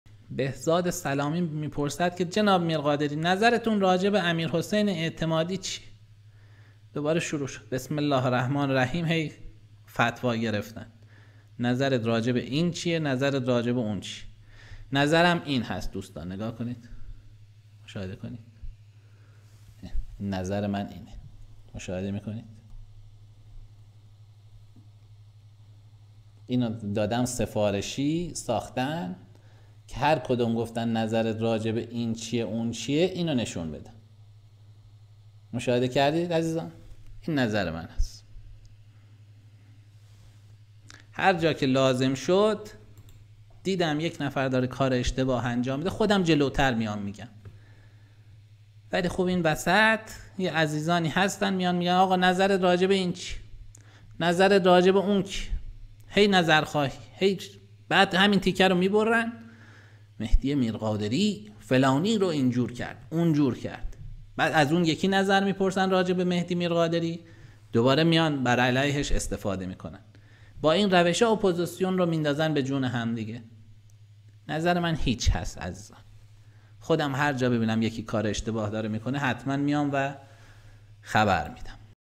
هیچ، پاسخ کسانی که میخواهند اپوزیسیون را به جان هم بیاندازند
پاسخ مهدی میرقادری درباره کسانی که میخواهند اپوزیسیون را به جان هم بیاندازند